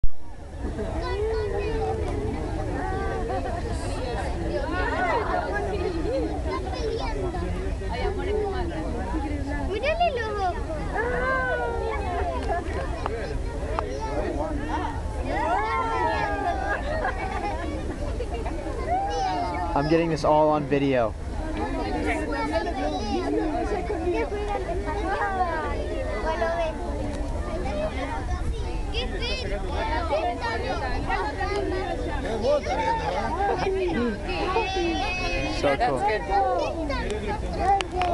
my white tigers
these are two white tigers i filmed in south america